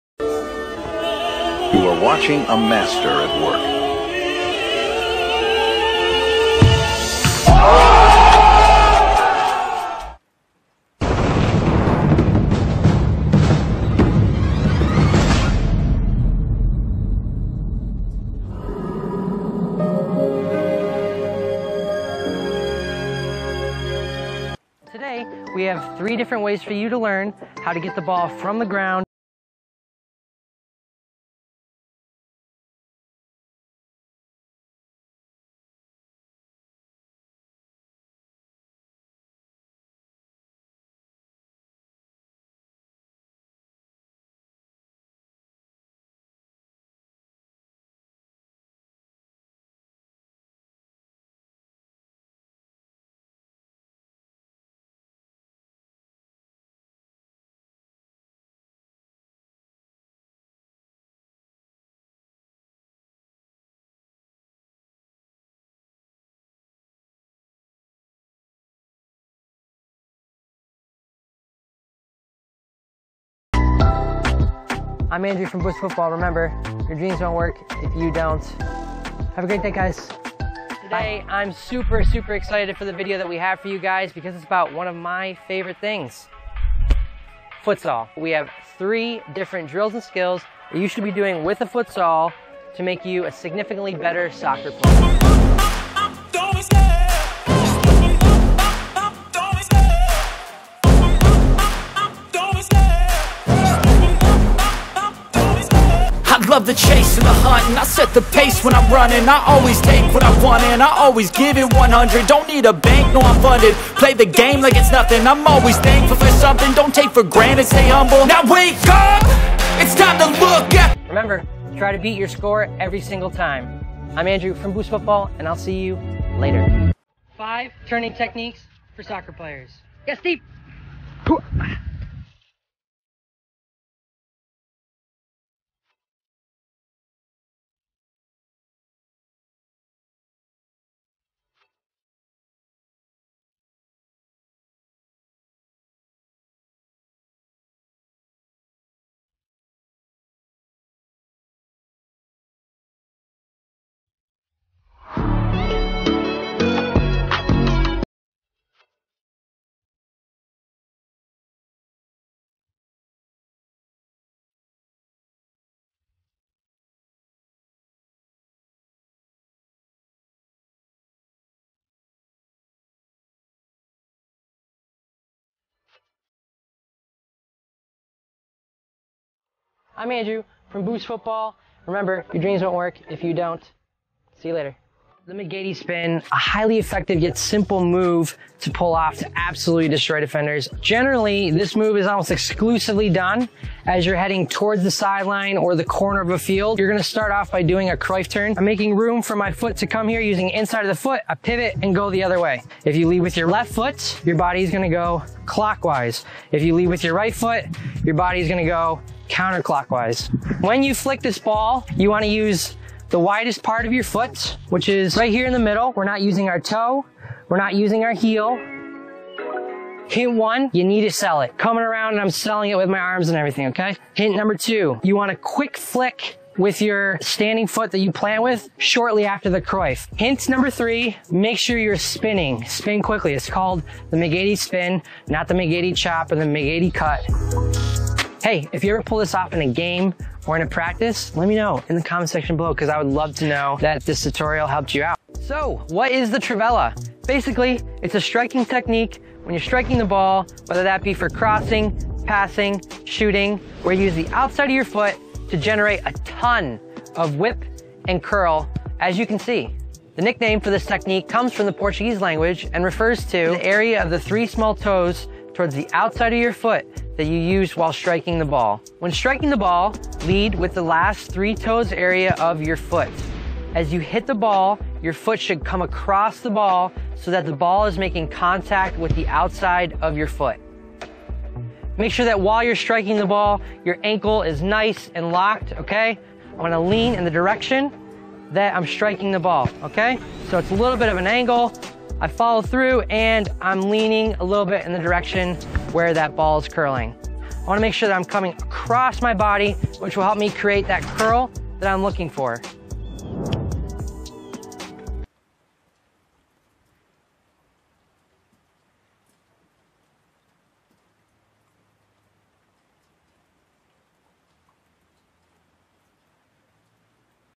Teknik Sepak Bola Untuk Pemula | Video Tutorial Football Skills
Siapa yang tidak ingin menjadi Greatest of All Time [GoAT] seperti halnya Lionel Andres Messi dan Cristiano Ronaldo dos Santos Aveiro.

Bagi kalian yang ingin memiliki skill seperti mereka, jangan pernah ragu dengan kemampuan yang kamu miliki, pelajari dan kuasi beberapa teknik di konten video ini.